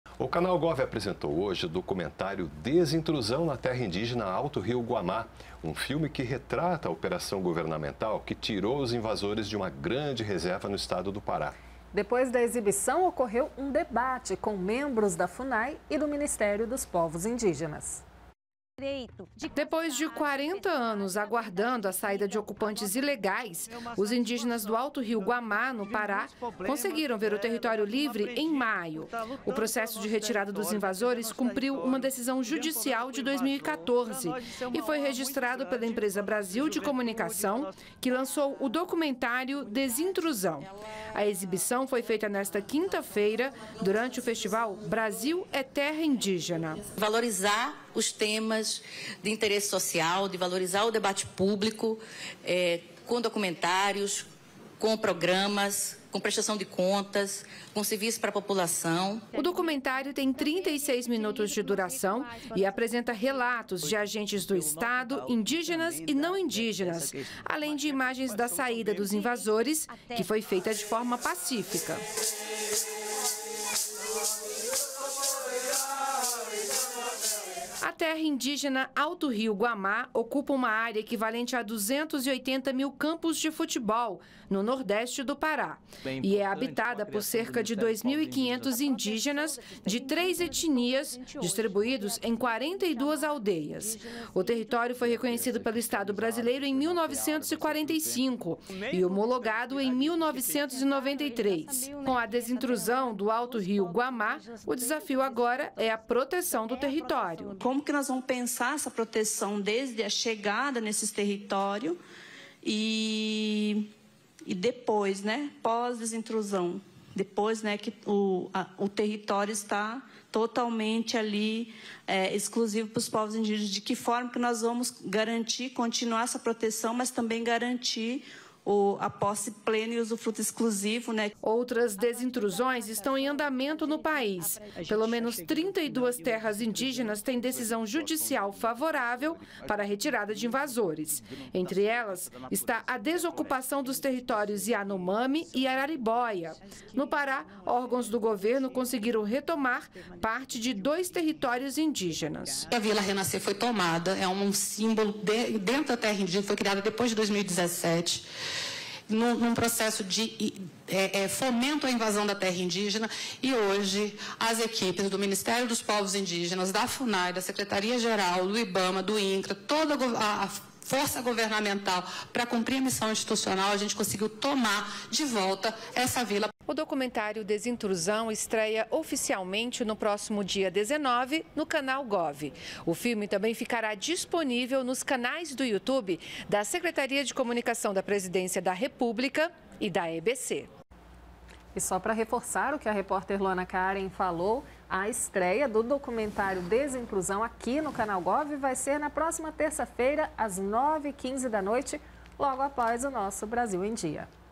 DOCUMENTÁRIO EXIBIDO PELO CANAL GOV É PANO DE FUNDO PARA DEBATE EM TORNO DA CAUSA INDÍGENA
O Canal Gov exibiu o documentário "Desintrusão na Terra Indígena Alto Rio Guamá", que tem como pano de fundo o debate em torno da causa indígena. A exibição foi seguida por um debate com membros da Funai e do Ministério dos Povos Indígenas.